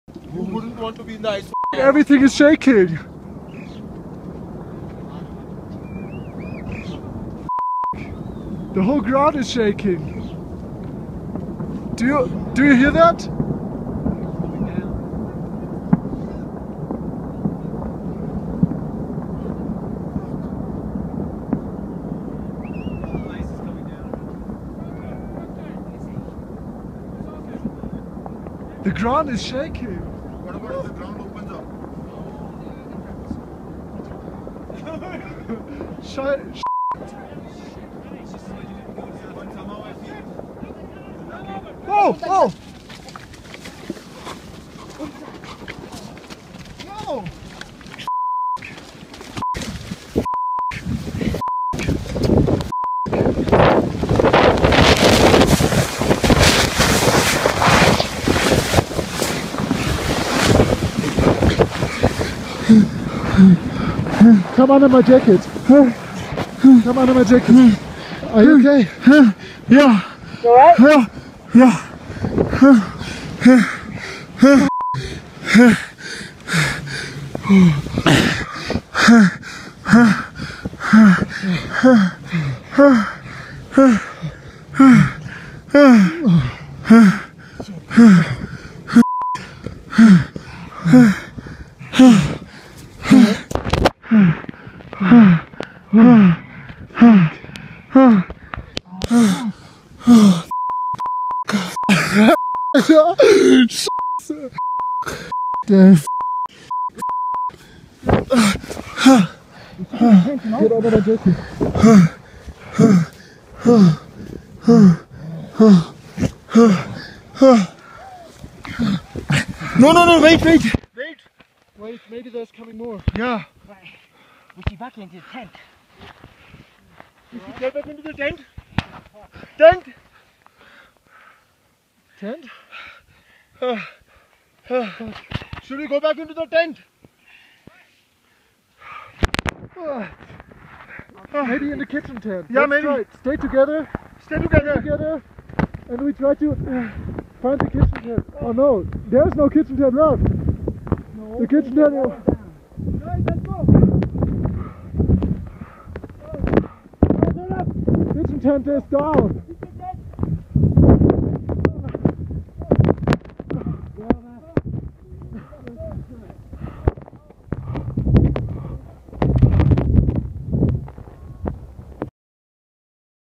Hit by avalanche in Everest basecamp 25.04.2015 FULL QUALITY - EXTENDED VERSION
This is the original file from my I phone in full Quality and full lengh which was to big to upload originally.

The ground was shaking from the earthquake and as soon as we saw people running we were running ourselves to save our lives.